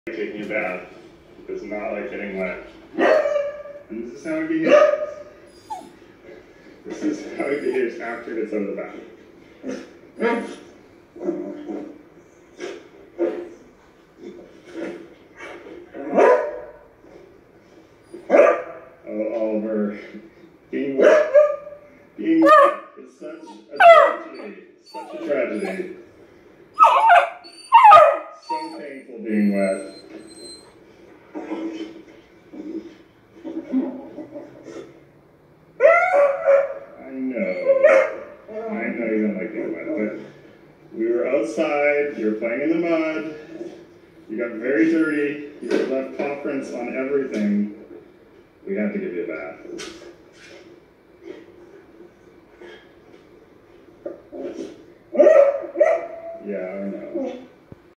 My beagle reaction watching Oliver the Beagle video
My beagle is wathing one of Oliver the Beagle's video and this is his reaction.

Here is the video my beagle is reacting to..